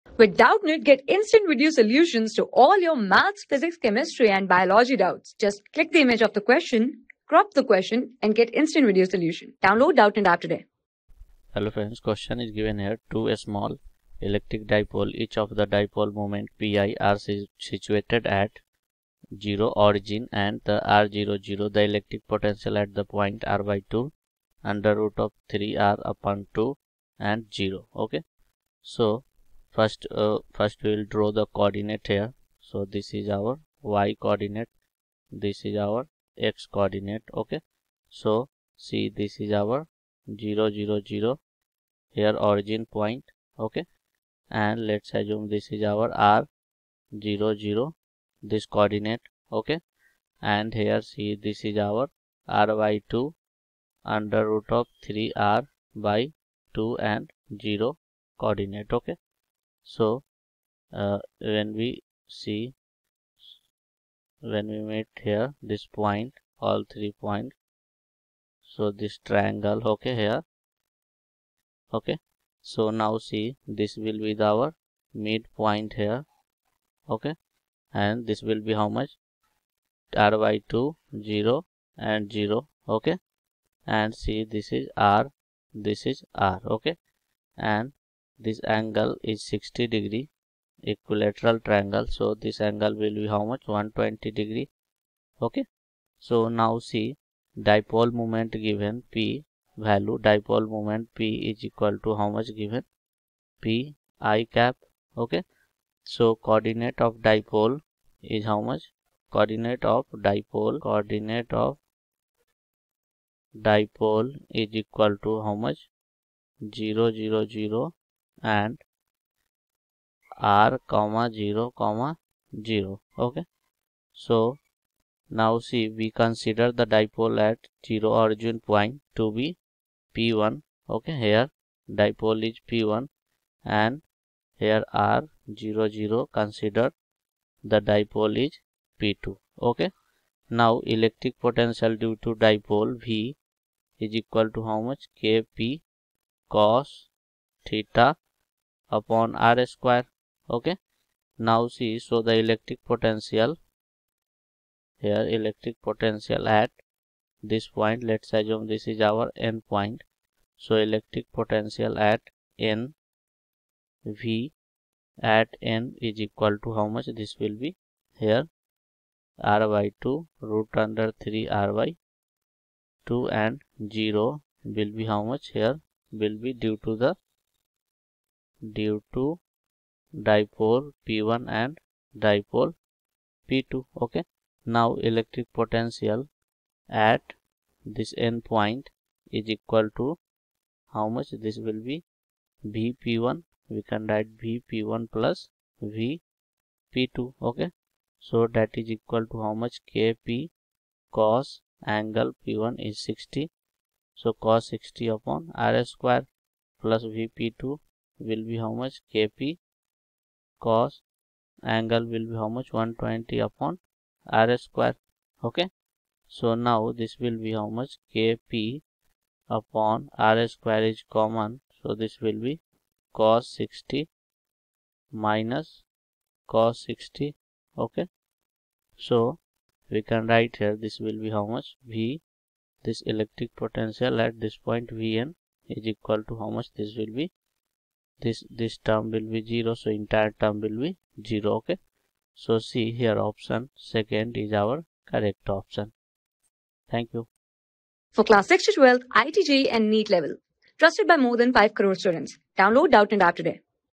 Two small electric dipoles each of dipole moment p hati are situated at (0,0,0) and (r,0,0). The...
Two small electric dipoles each of dipole moment p hati are situated at (0,0,0) and (r,0,0). The electric potential at a point ((r)/(2),(sqrt(3r))/(2),0) is:
Class: 12
Subject: PHYSICS
Chapter: DAILY PRACTICE PROBLEM
Board:IIT JEE

👉 Have Any Query? Ask Us.